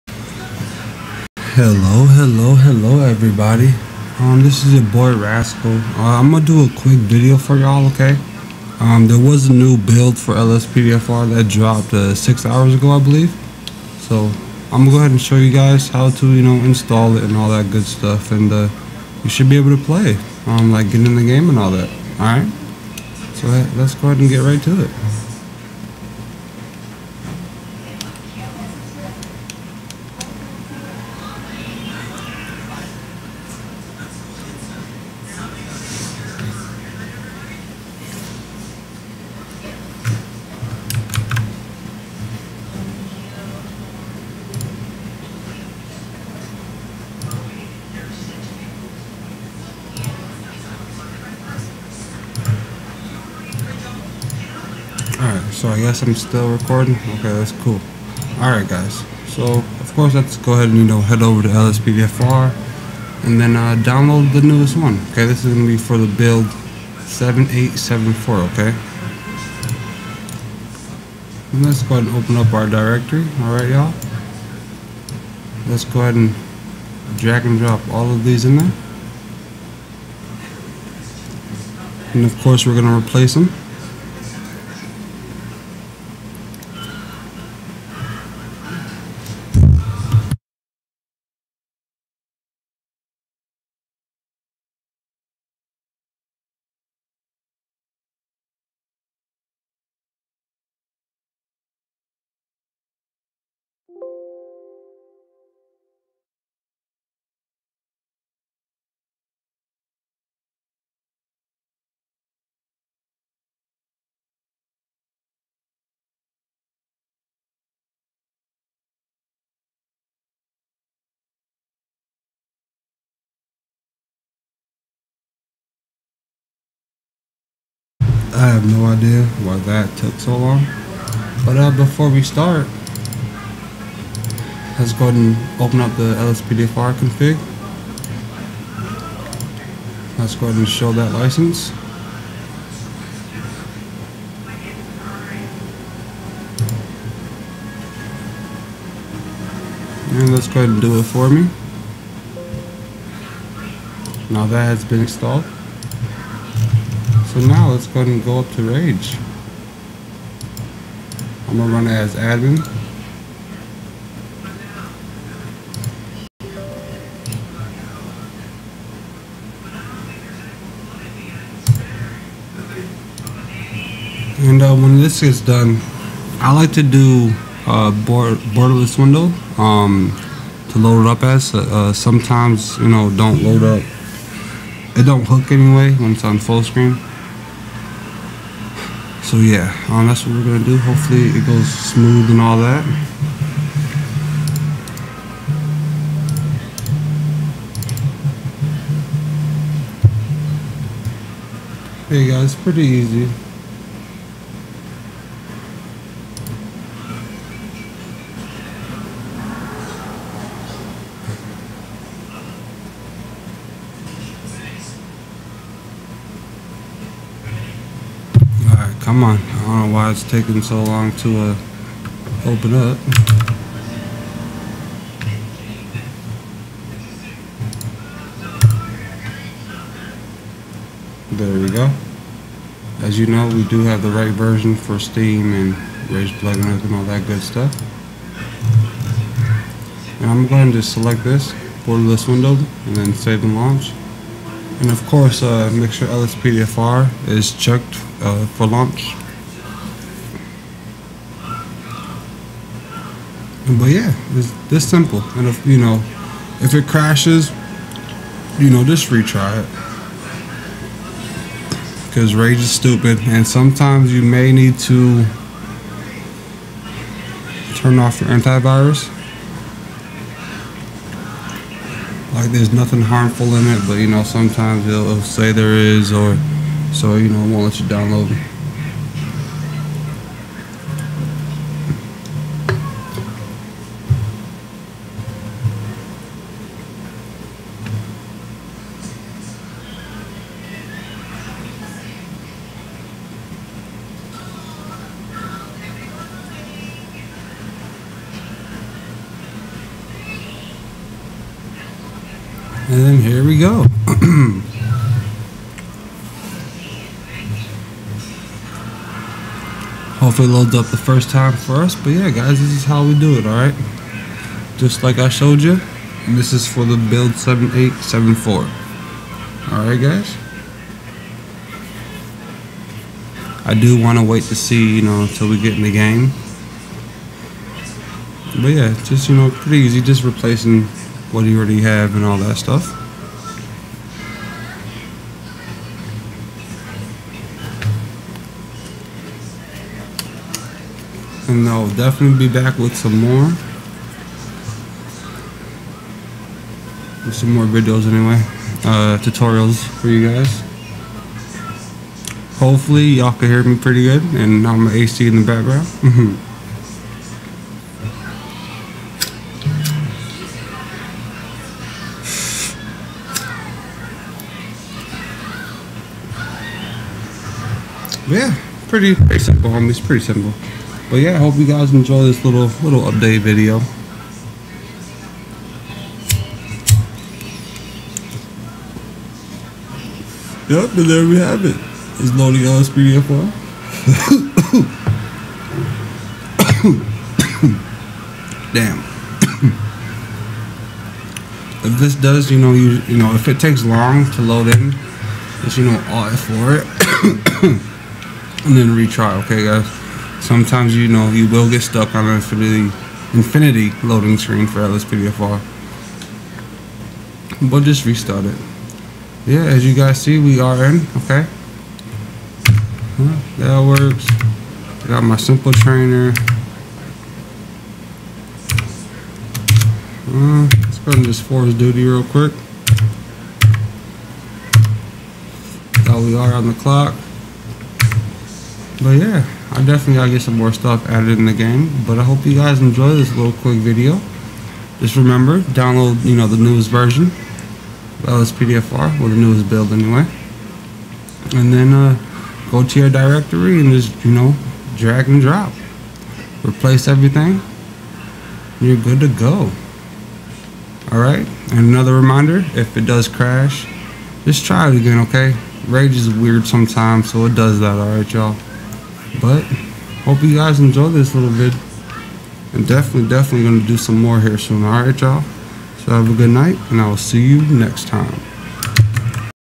How to Install the newest LSPDFR 0.4.9 (Build 7874)
Here’s a little update video since they came out with a little build update hahaha
Enjoy the videos Homies!